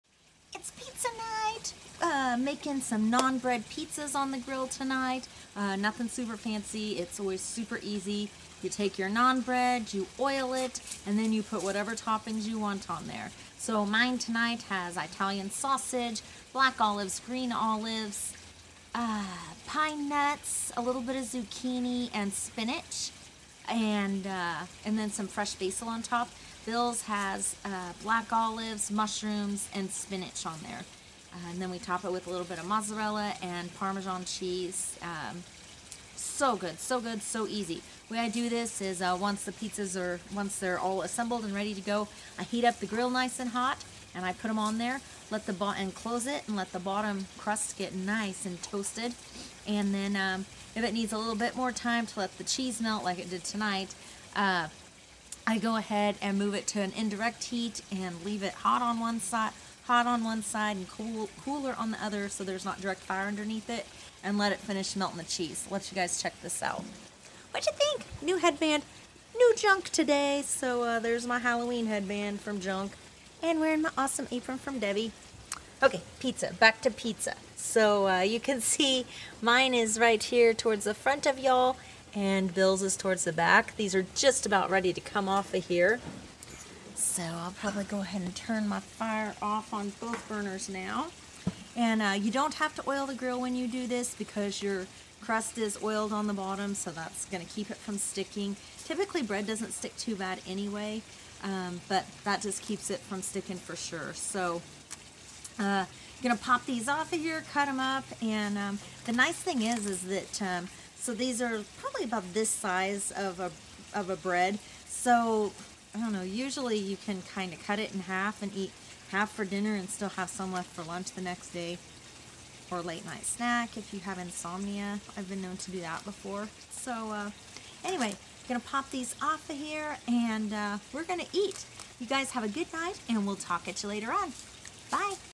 Naan Bread Pizza Night
Keeping dinner simple tonight and grilling up naan bread pizzas!